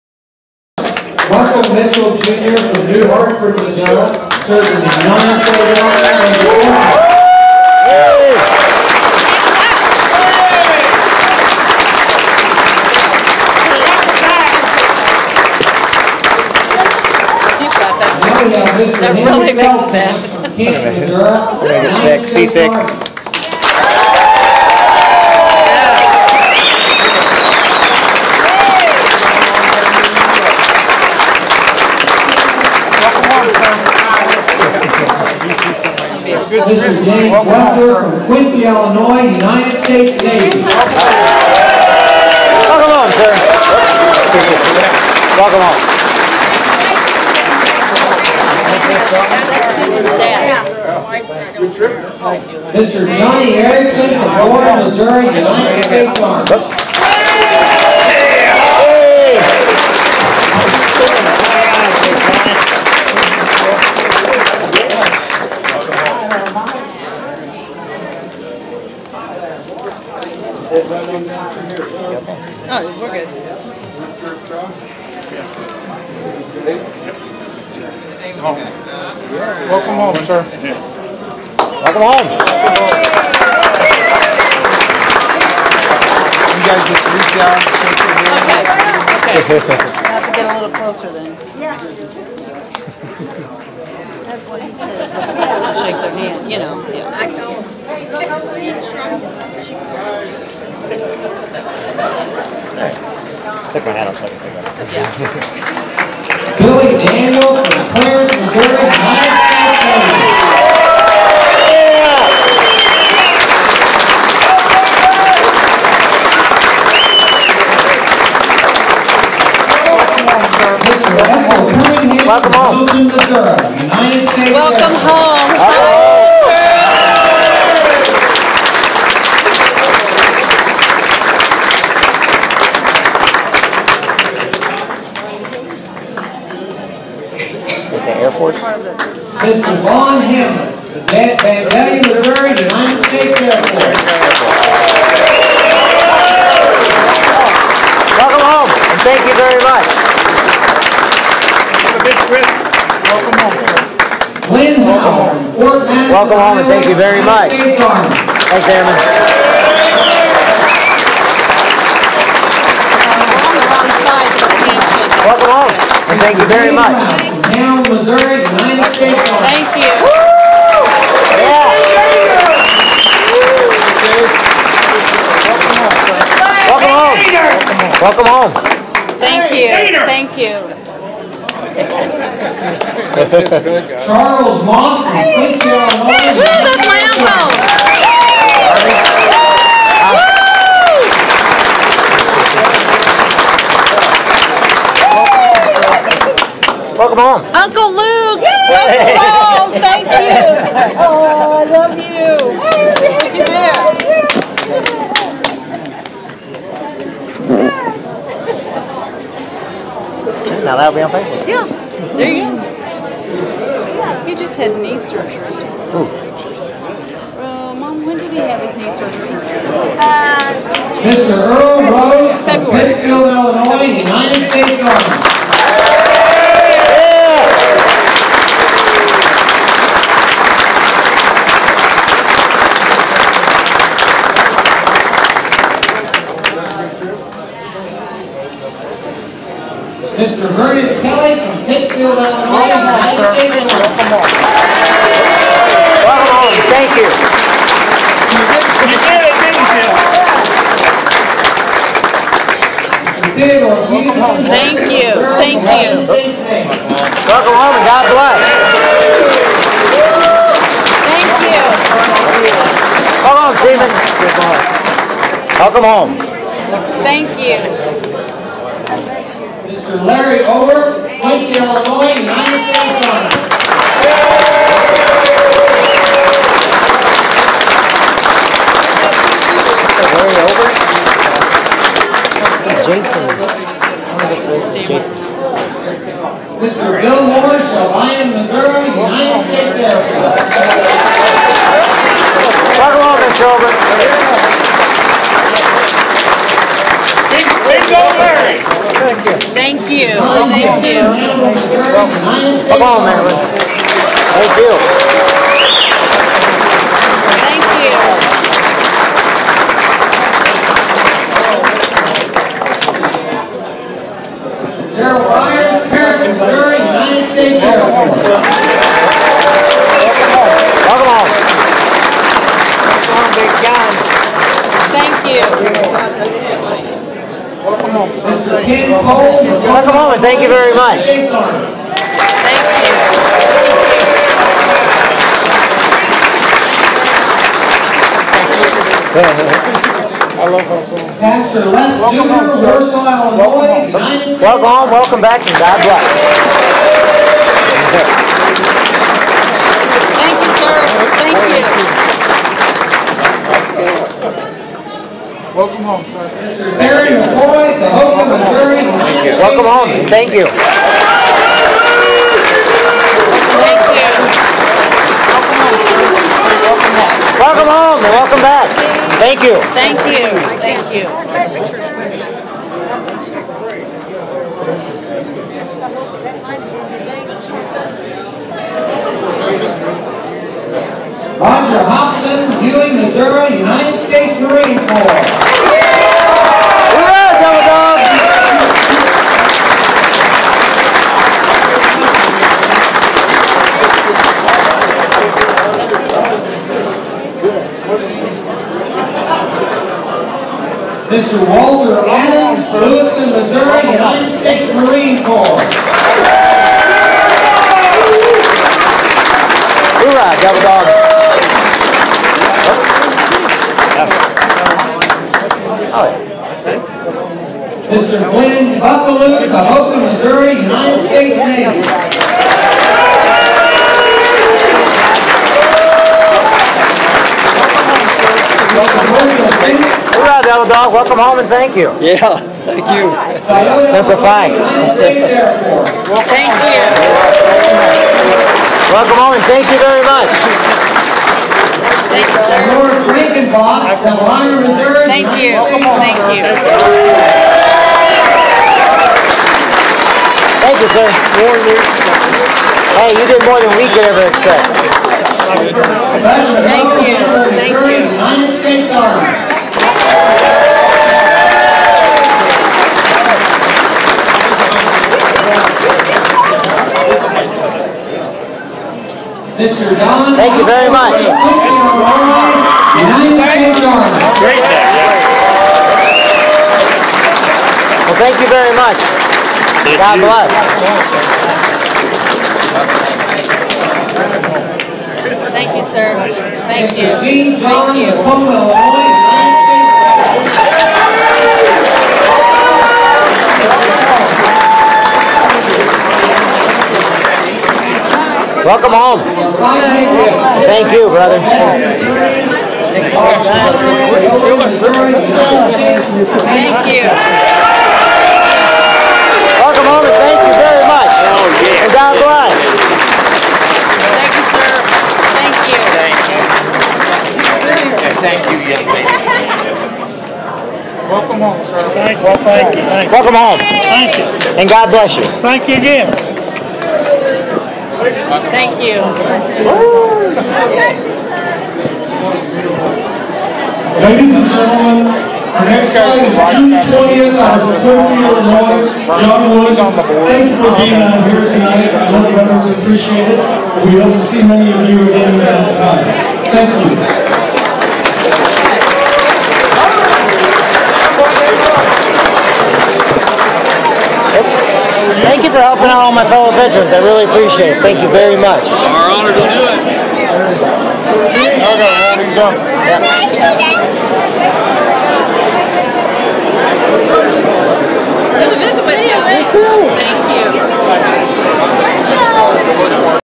May 9 2013 Great River Honor Flight The real heroes are welcomed home. Red carpet video
My fellow brothers my fellow veterans